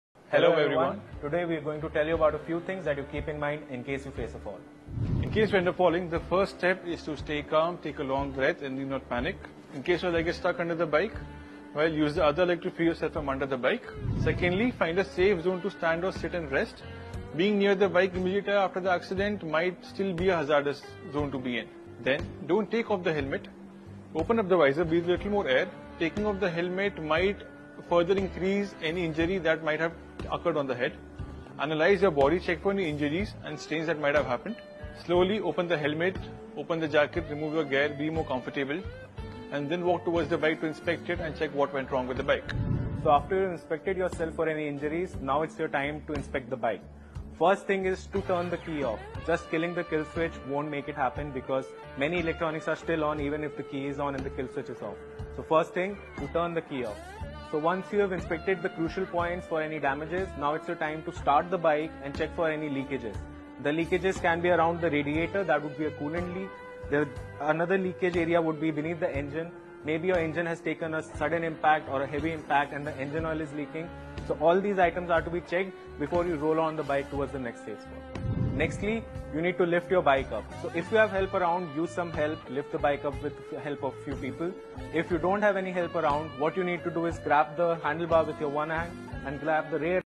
Staying Safe During a Fall: Tips and TricksIf you ever find yourself unfortunate falling situation
the best thing to do is stay calm, take a deep breath and remember these helpful tips. Don’t panic if your leg is stuck, use your other leg to free yourself from the bike. Once you’re safe, make sure you find a secure area to stand on.